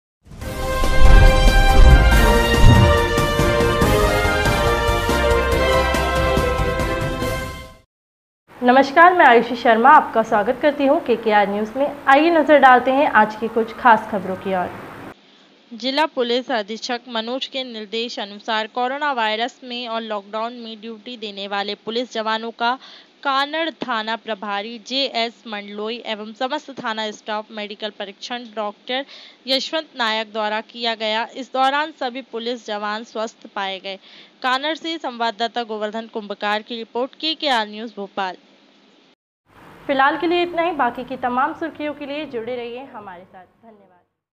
*पुलिस जवानों का मेडिकल परीक्षण कराया गया*
कानड़ :- जिला पुलिस अधीक्षक मनोज के निर्देश अनुसार कोरोना वायरस मै ओर लाँकडाउन मैं ड्यूटी देने वाले पुलिस जवानों का  कानड़ थाना प्रभारी जे एस मंडलोई एवं समस्त थाना स्टाफ  मेडिकल परीक्षण डॉ यशवंत नायक द्वारा किया गया इस दौरान सभी पुलिस जवान स्वस्थ पाए गए

कानड़ से संवाददाता गोवर्धन कुम्भकार की रिपोर्ट